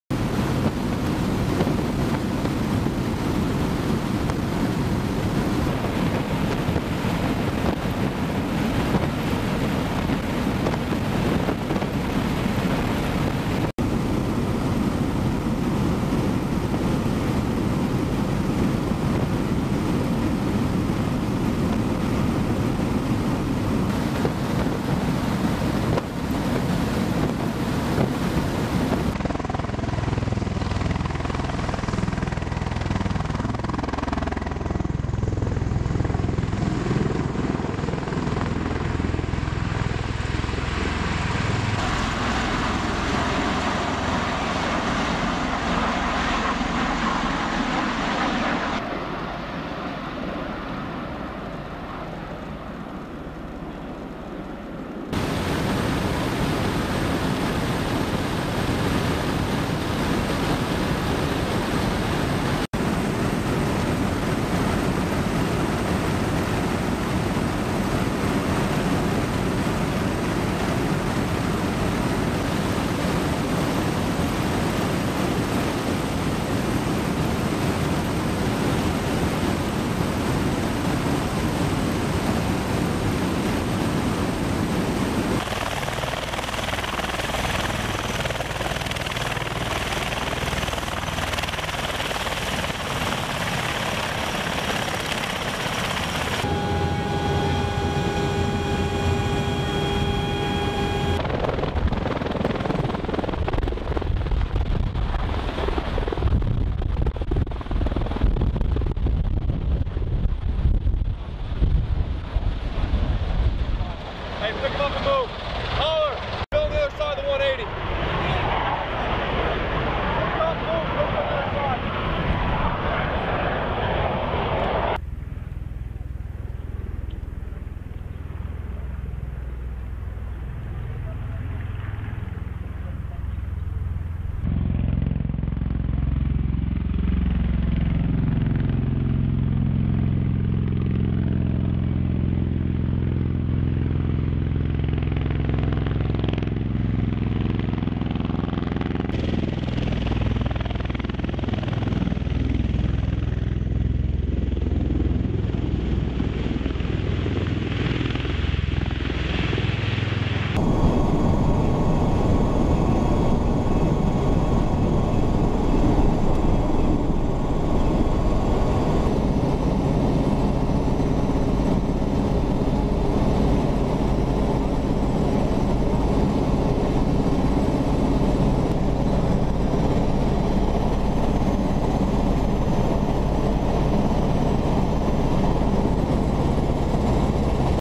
Ospreys in Romania: USMC MV-22s Join Multilateral Exercise - OM 4537
Raw footage for those Osprey junkies out there!

U.S. Marine MV-22B Ospreys from Special-Purpose Marine Air-Ground Task Force Crisis Response-Africa departed Mihail Kogălniceanu Air Base, Romania, with a platoon of Marines from the Black Sea Rotational Force to support a multilateral training exercise during Platinum Eagle 15, May 26, 2015.

The Ospreys left Morón Air Base, Spain, earlier in the week, and arrived to work with their allies from the Bulgarian and Romanian armed forces. The Ospreys delivered the Marines to the Babadag Training Area, where they demonstrated their ability to conduct security operations.

The training represents the first time Ospreys have conducted multilateral training in Romania, where members of BSRF have established a growing relationship with partner Romanian forces, which is crucial in the growing movement of US and NATO forces to deter Russian forces from aggressive moves in southern Europe this summer.

This work by Sgt Paul Peterson is free of known copyright restrictions under U.S. copyright law.